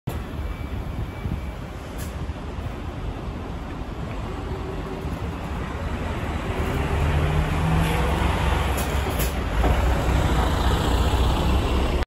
【両備バス】山陽道を西へ！1715 サンサンライナー広島BC行 岡山駅発車
岡山駅16:10発 広島バスセンター18:52着 高速バスサンサンライナー広島行
両備バス1715